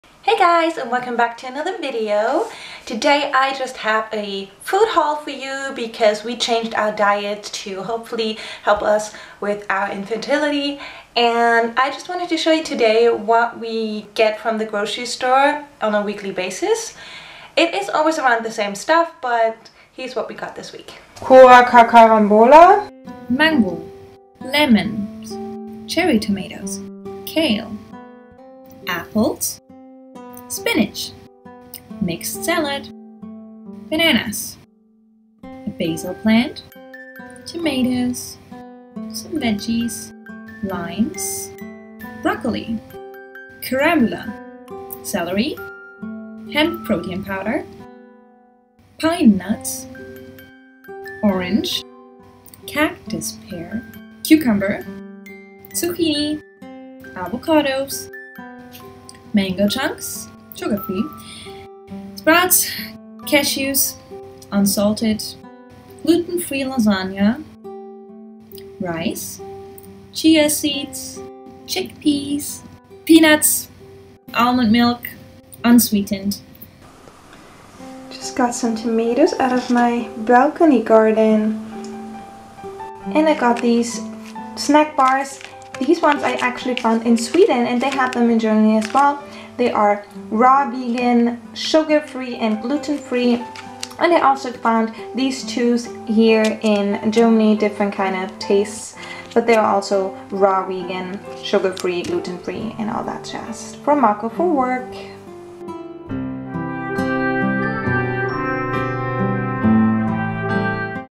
Grocerie Haul // Healthy Fertility Diet
3 months into our vegan journey and we are feeling great. Here is a Food Haul of our weekly purchases.

In This Vlog:
Annika and Marco have been married for 1 year!

I own all the footage and content of my videos. Please do not reproduce without my permission.